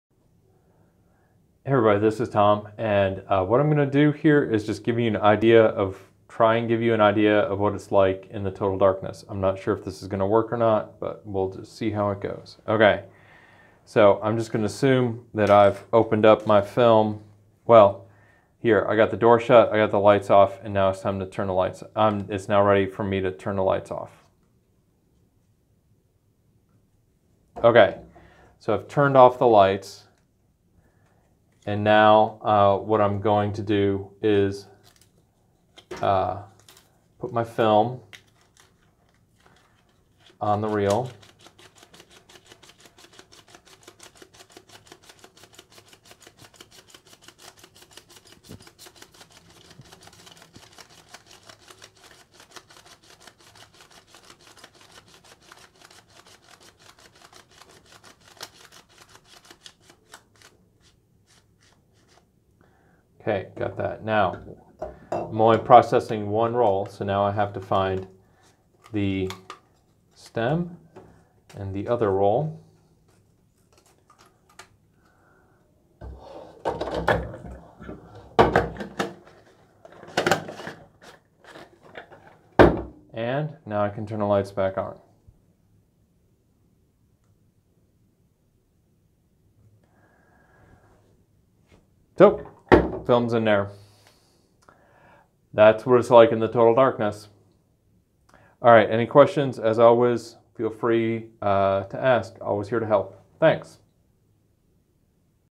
Loading 35mm Film In the Dark
This video shows what it is like to load film onto plastic developing reels in total darkness. I start by arranging what I need on the counter, then turn off the lights and load the film onto the reels, and put the reel with the film on the bottom of the developing tank.

THIS PROCESS MUST HAPPEN IN A CHANGING BAG OR TOTAL DARKNESS.

This video was created for the Photography 1 class I teach at the University of Wisconsin Oshkosh. If you find that this video would help you, please use and share it.